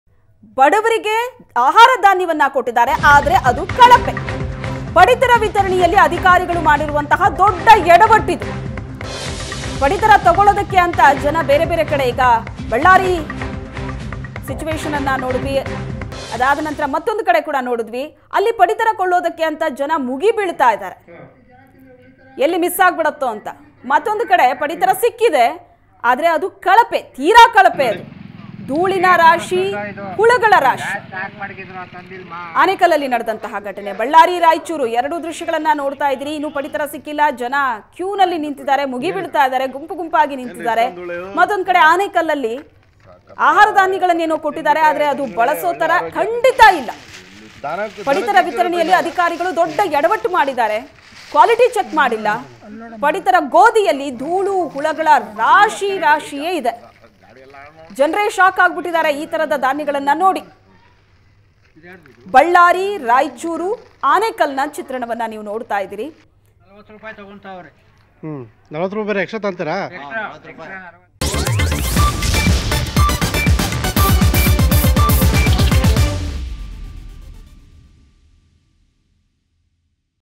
COVID19: PDS Beneficiaries Stand In a Queue Before a Ration Depot in Bellary & Raichur
Karnataka witnessed its highest single day jump in coronavirus cases as 16 more people, including a radio jockey in Bengaluru, tested positive. The state’s tally now stands at 144.